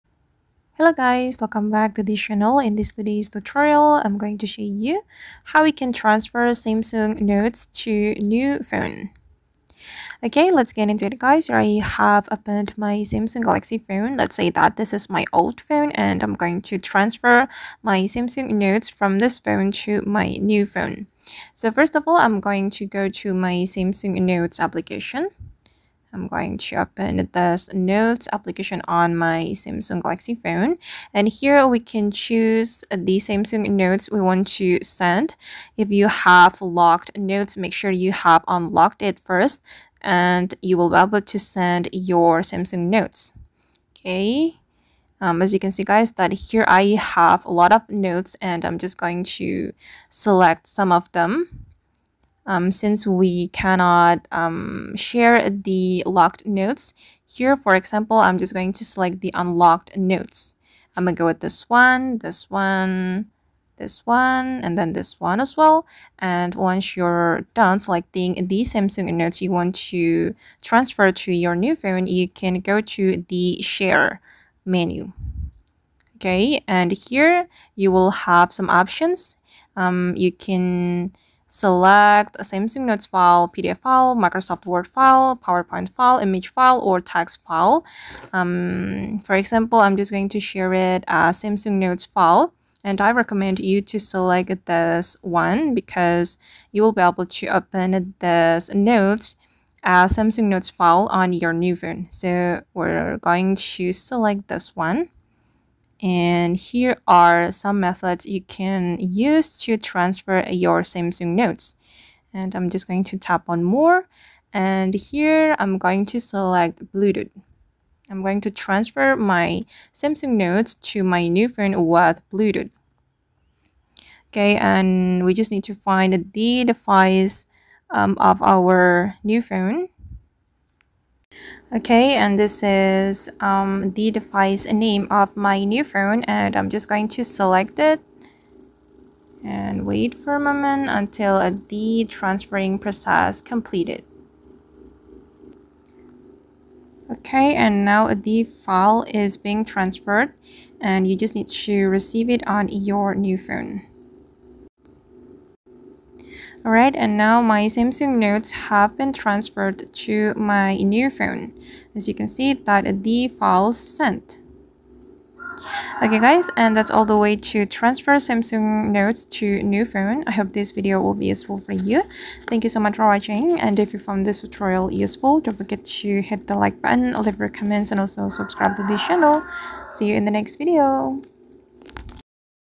💲 TRAINING: HOW TO TRANSFER SAMSUNG NOTES TO NEW PHONE (Easy Way) | Full Tutorial
Got a new Samsung phone and want to transfer all your Samsung Notes easily? In this full tutorial, I’ll walk you through the simple steps to move your notes to your new device without losing any data. This method works great for keeping all your important information in one place, making the switch to a new phone stress-free. Perfect for anyone upgrading or switching devices. Let’s get started and keep your notes safe!